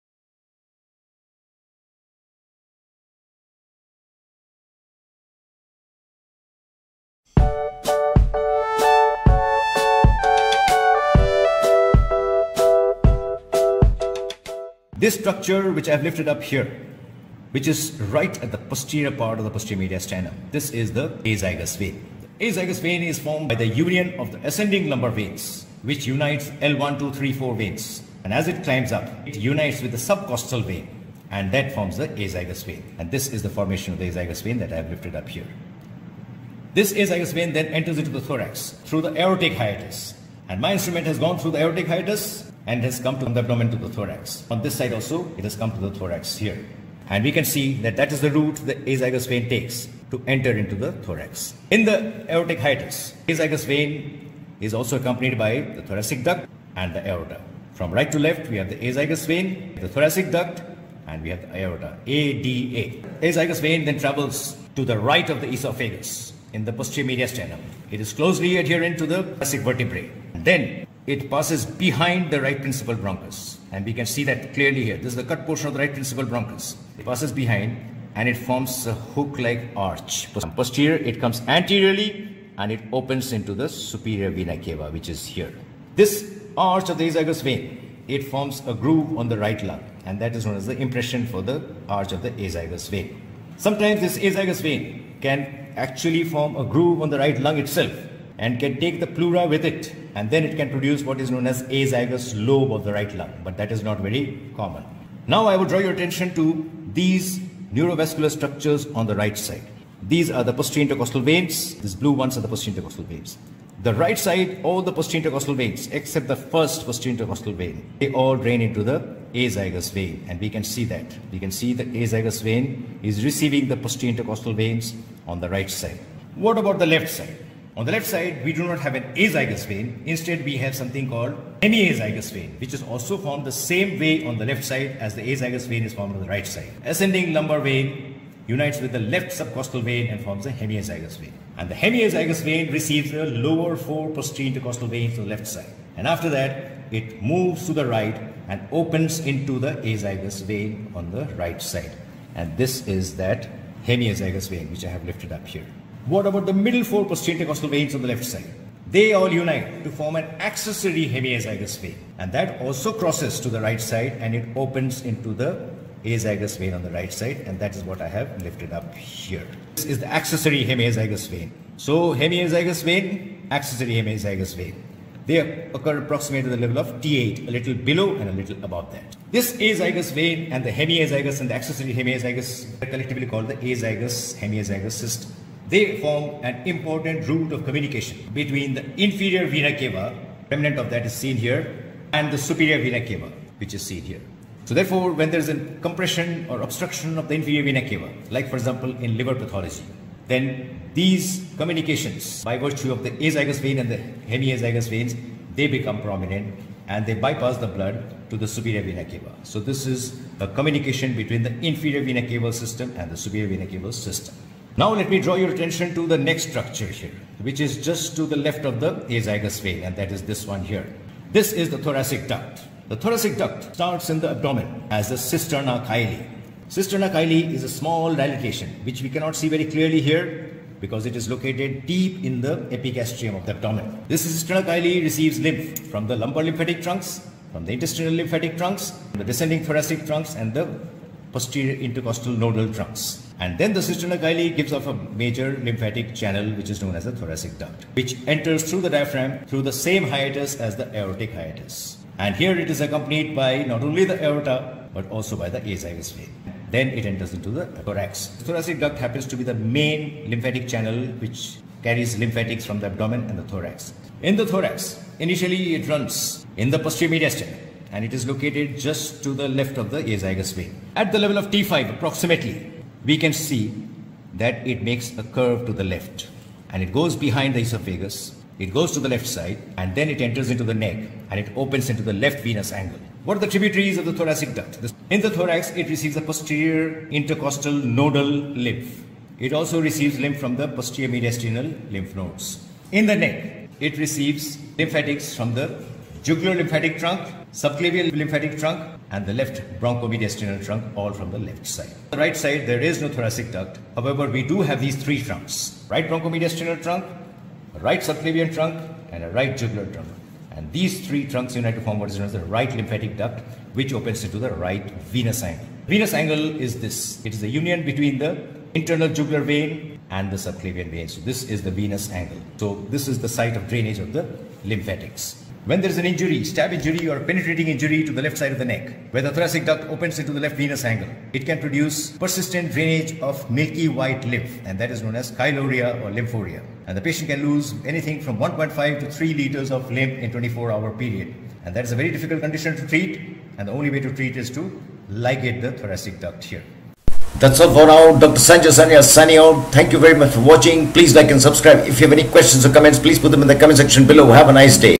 AZYGOS HEMIAZYGOS ACCESSORY Hemiazygos THORACIC DUCT and Clinicals – Sanjoy Sanyal – Proceum
This video shows the dissection of the Azygos Vein, Thoracic Duct and Vagus. The specimen was harvested from a cadaver.
PLENTY of clinical correlations are described, Esophageal Varices, Lymphorrhea, Azygos fissure, Azygos Lobe etc.
Cameraperson was David O., using my Samsung Galaxy S8 with its 8 MP rear and 12 MP front-facing cameras. Your acquiescence in this regard is much appreciated!